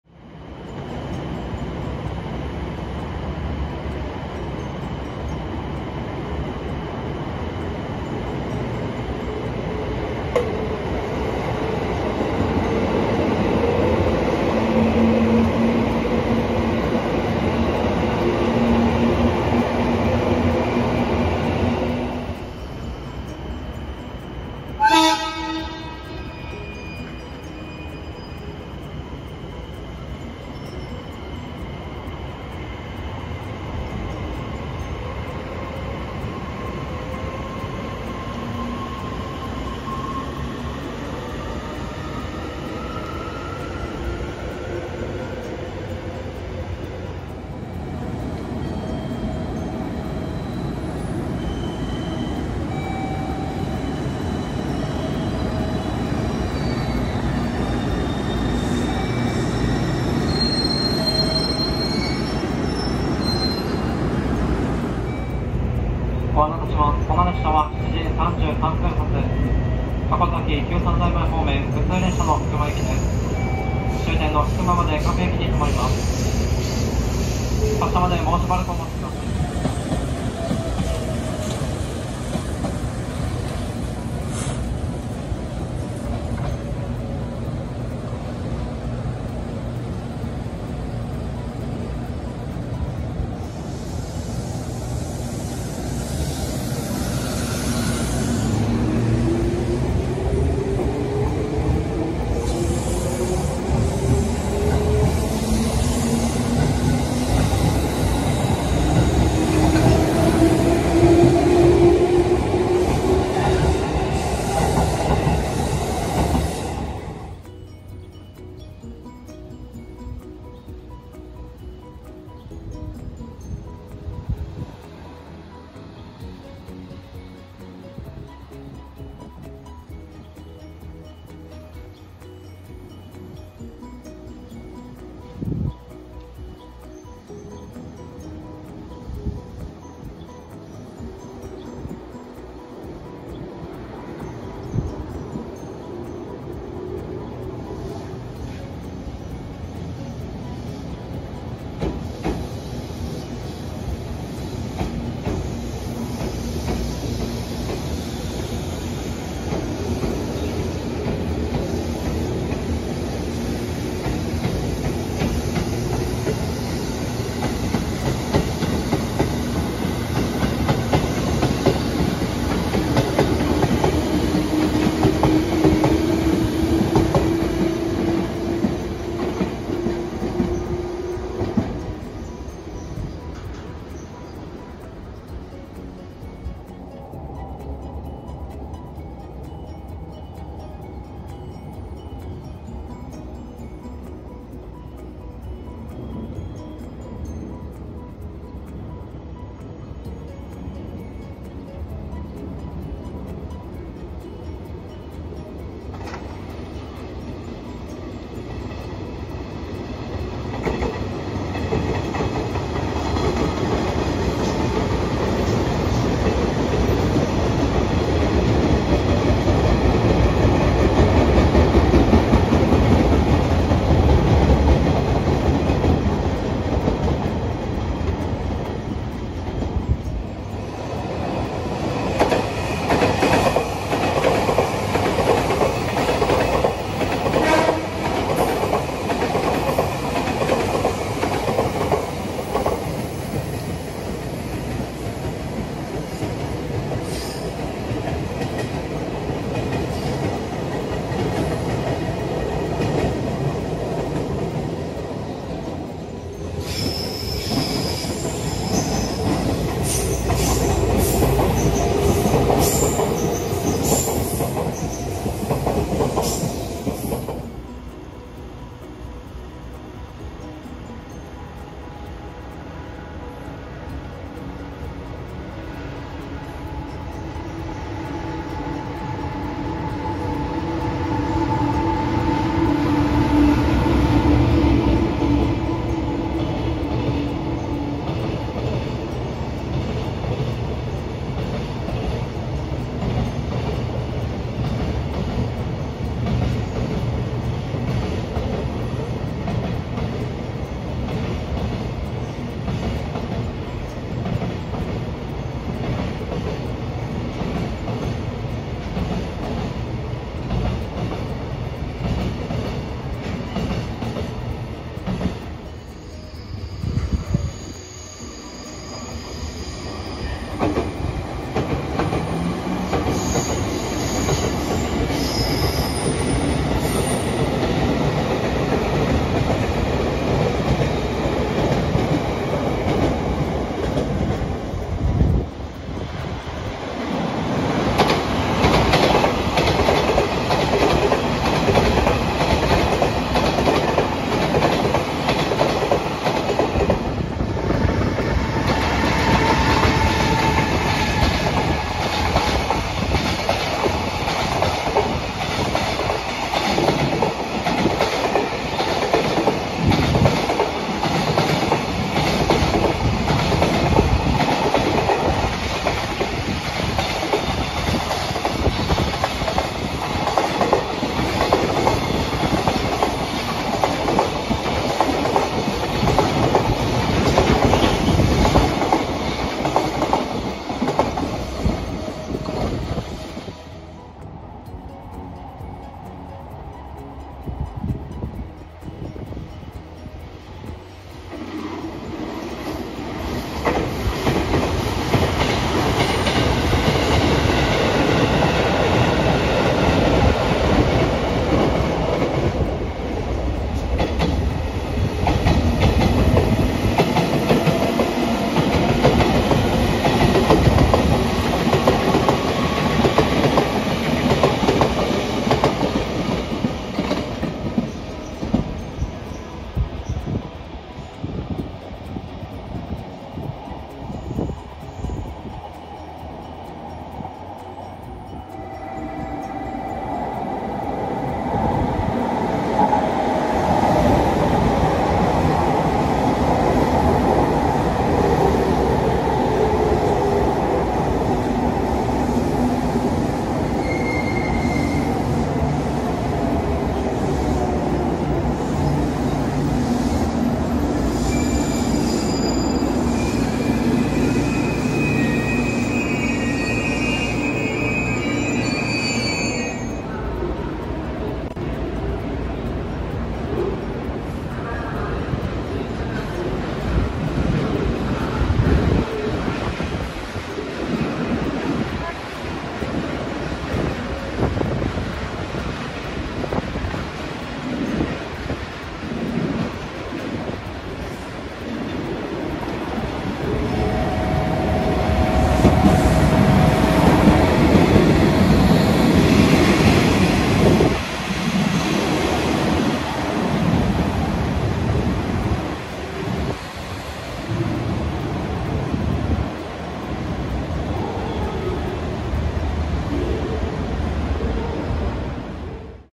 ６／23の撮影あれこれ📸😆jr九州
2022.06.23
通勤途中の博多駅や千早駅、職場から撮影しました😃